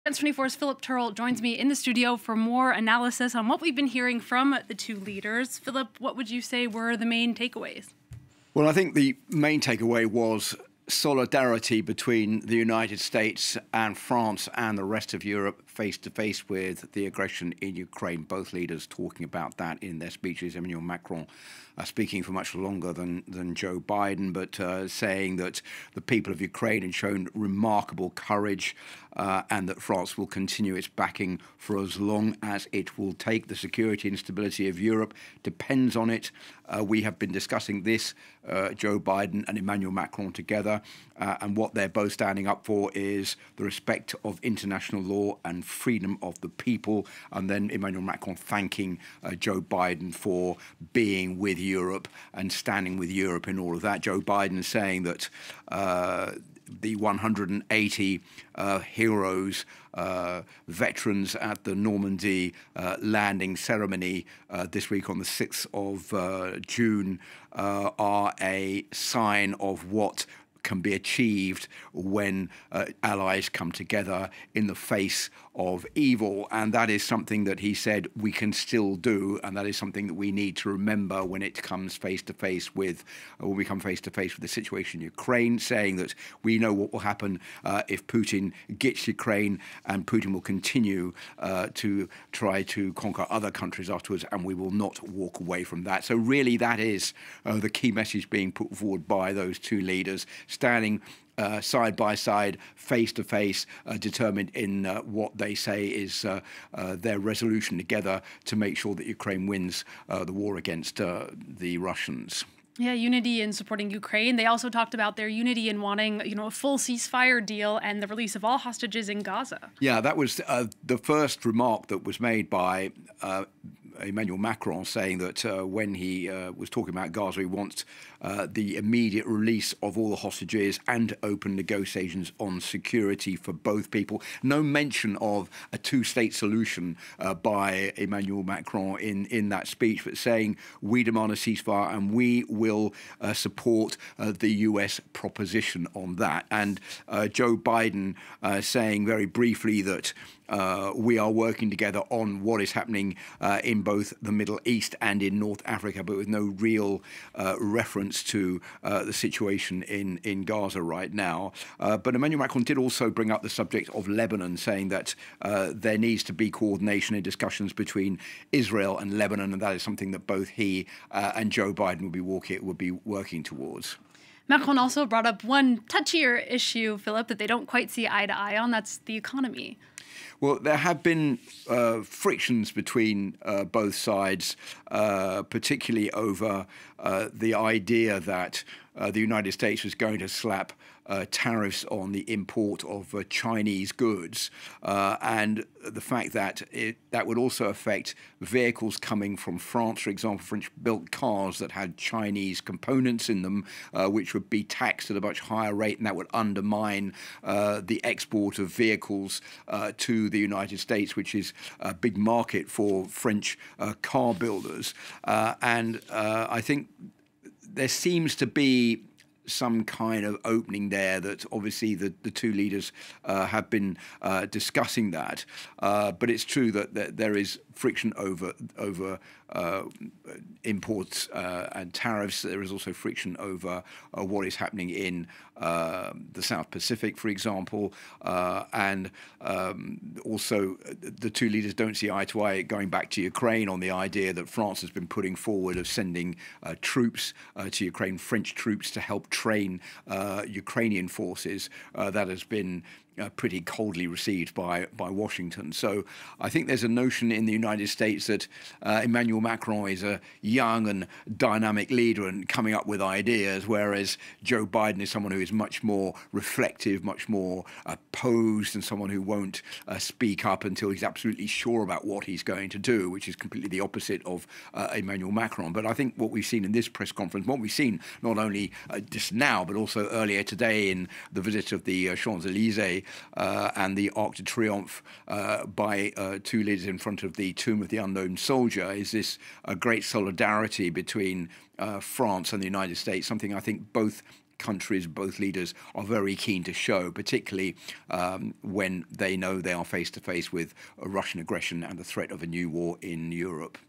Macron-Biden presser: 'Solidarity' with Ukraine against Russian aggression • FRANCE 24 English
One of the main takeaways from French President Emmanuel Macron and US President Joe Biden’s joint press conference on Saturday at the Elysée Palace was “solidarity between the United States and France and the rest of Europe face to face with the aggression in Ukraine,” FRANCE 24 International Affairs Editor Philip Turle said in an analysis. The two heads of states voiced their support for Ukraine, emphasising the respect of “international law” as well as the “freedom of the people”, Turle said.